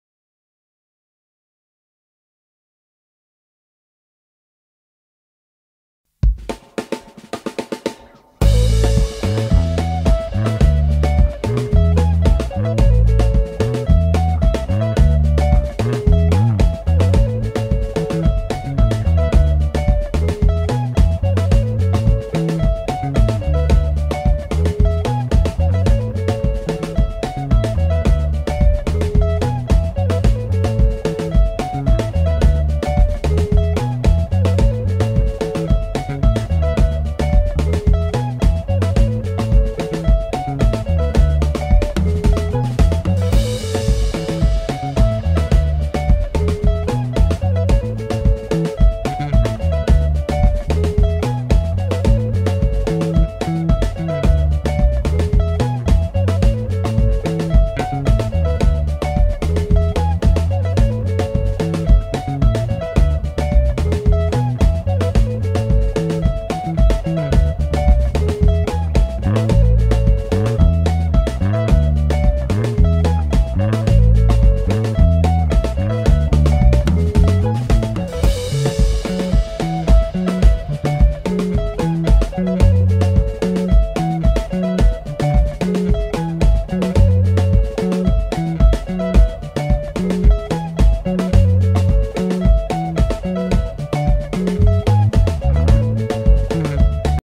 SEBEN LINE FOR BASS PLAYERS
Simple seben bass line in the key of A and a common seben chord progression(1-5-4-5)..